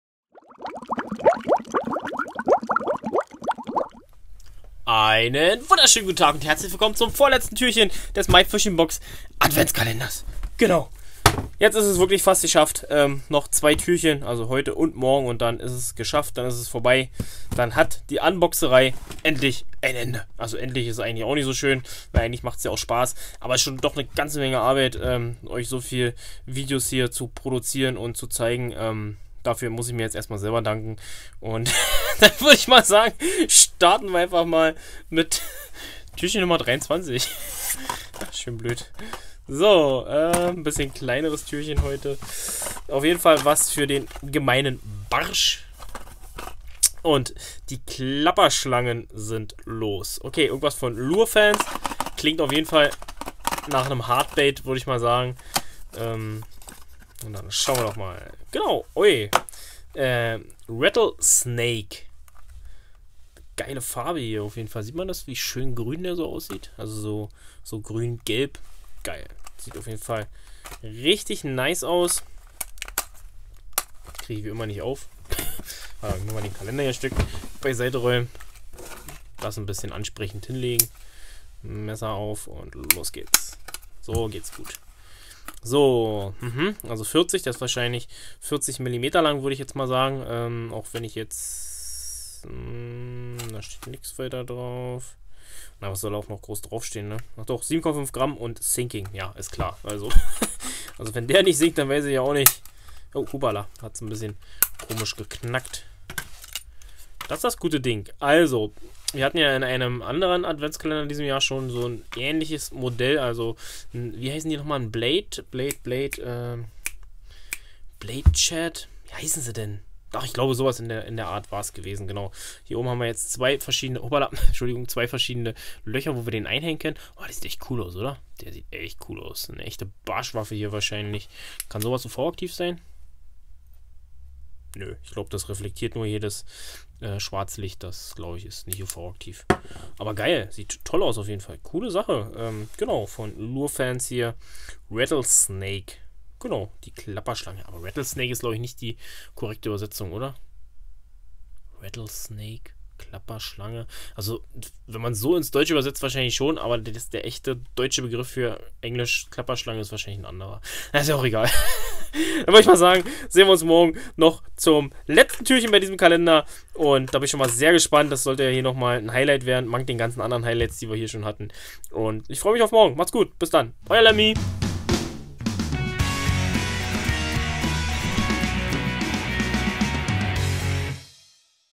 Fisch verliebt - MyFishingBox Adventskalender! Türchen Nummer 23! Cooles Teil!
Endlich Weihnachten, da wird es doch wieder Zeit, täglich ein Türchen zu öffnen! Viel Spaß und eine schöne Adventszeit!

Mein Equipment:
iPhone 11
Sony Alpha
Apple iMac 27"

Vielen Dank, Lemmi